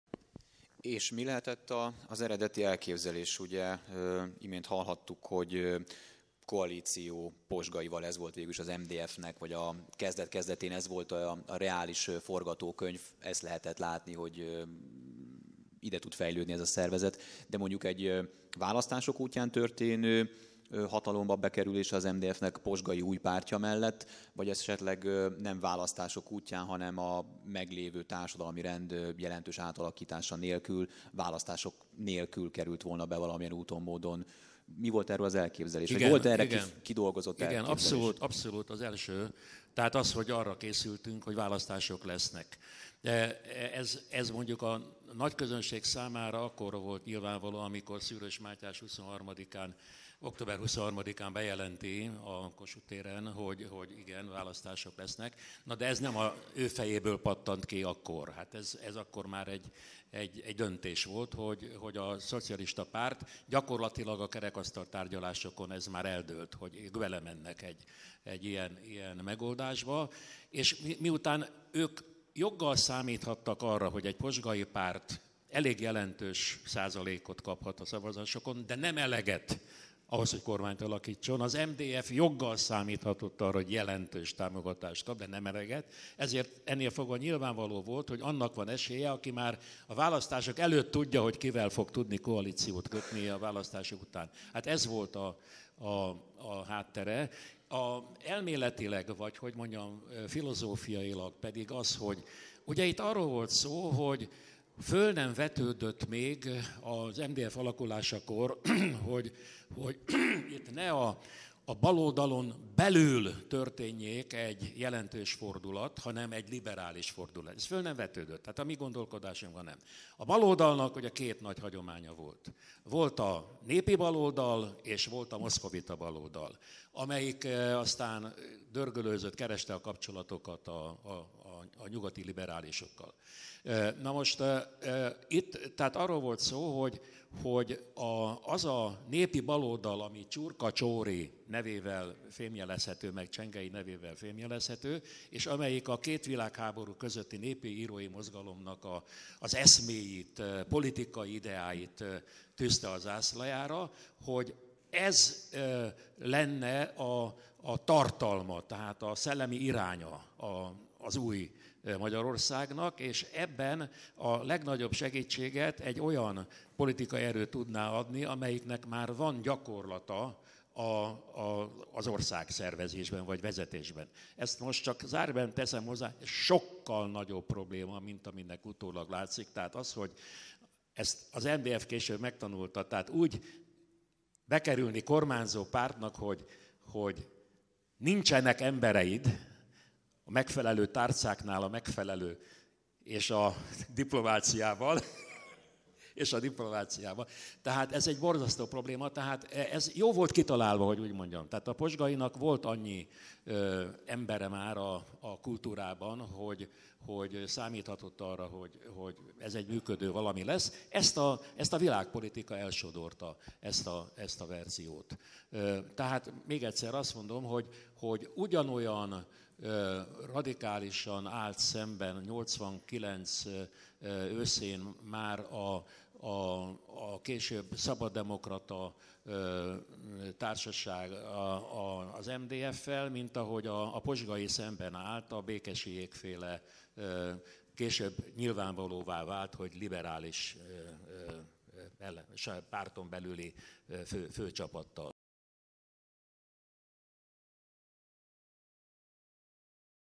Kulin Ferenc az MDF eredeti céljáról
"Ezt most csak zárójelben teszem hozzá sokkal nagyobb probléma, mint aminek utólag látszik - ezt az MDF később megtanulta -, úgy bekerülni kormányzópártnak, hogy nincsenek embereid a megfelelő tárcáknál és a diplomációban."

Dr. Kulin Ferenc a Pozsgay és az MDF eredeti céljáról és a kormányzóképesség személyi feltételeiről. A Modell és Valóság előadássorozat IX. évadának "Újratervezés -  Az MSZMP XIV. Kongresszusa történéseinek hatásai a MDF II. Országos Gyűlésére" című rendezvényére 2019. november 14-én került sor, Lesti Árpád moderálása mellett.